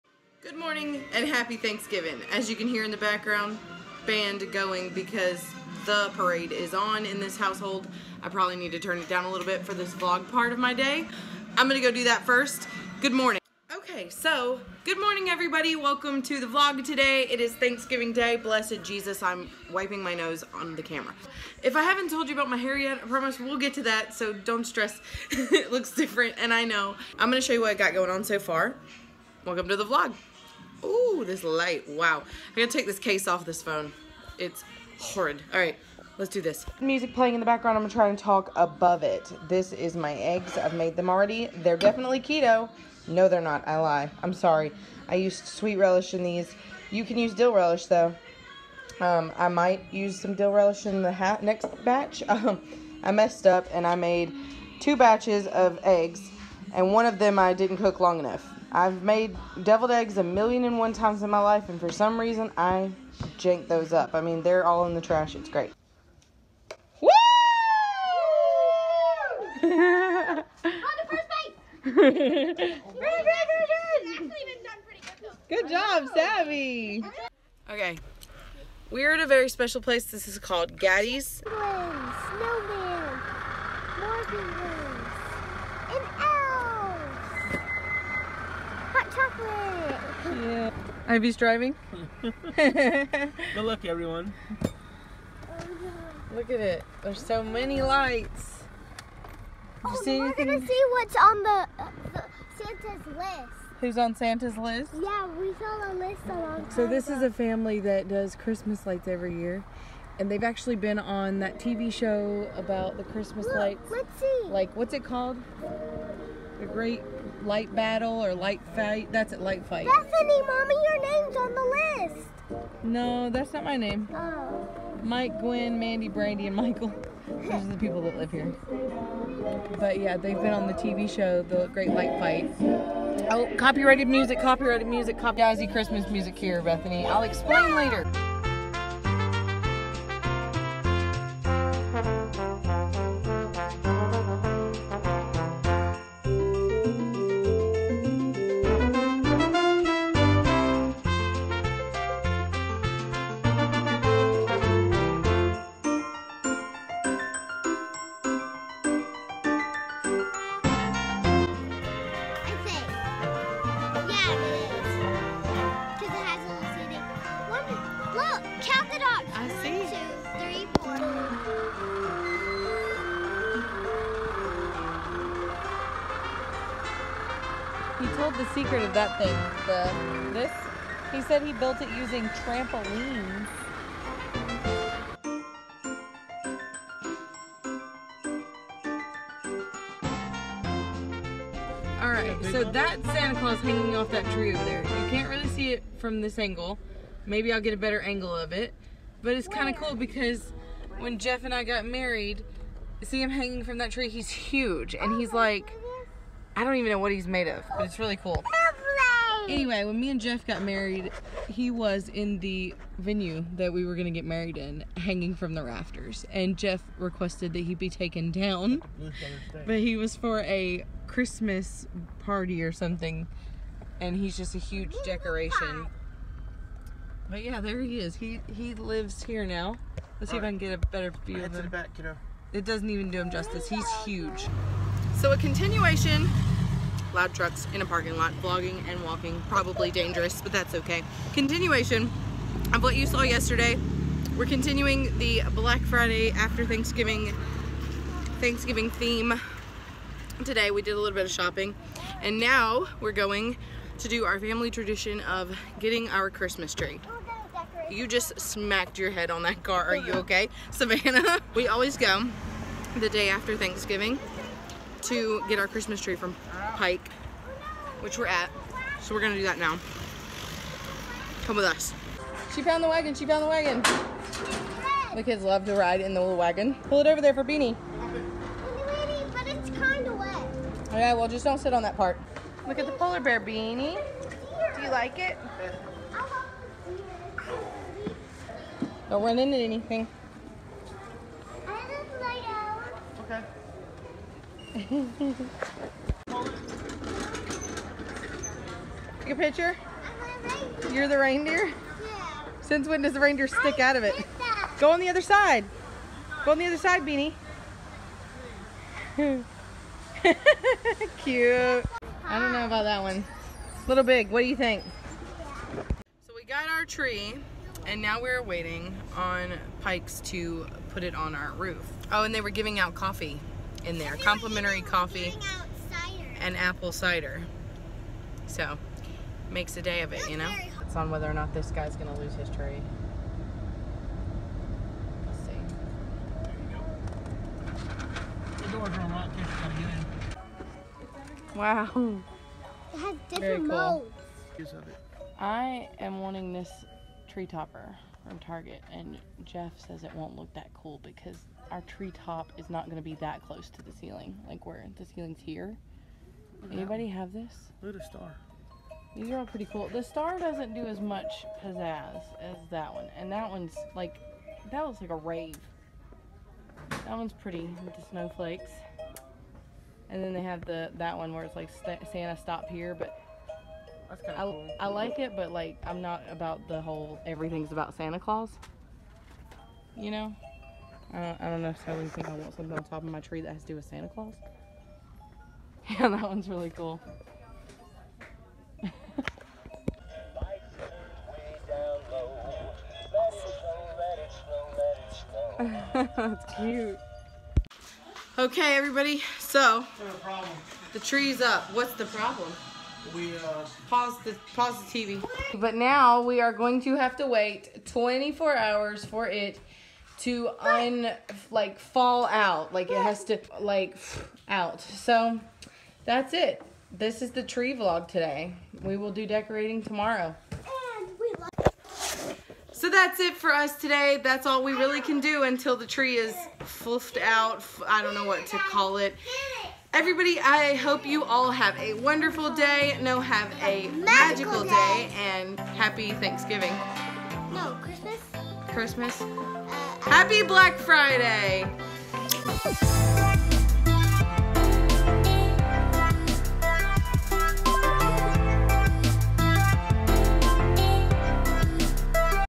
Thanksgiving & Black Friday VLOG| Come on our FAMILY TRADITION w/ us!
We go to thanksgiving dinner, then to see " the lights", and get our tree on Black Friday! come along!